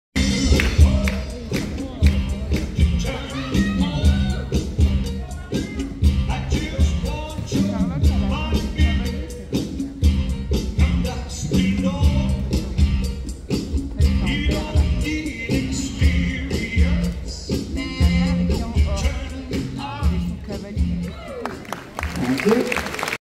CHA CHA CHA - Tiziano DI LOSA & Emanuela FELACO - Nuit de la danse 2024
Cha Cha Cha performance of Tiziano DI LOSA & Emanuela FELACO at Nuit de la danse et de l'élégance on February 3rd, 2024.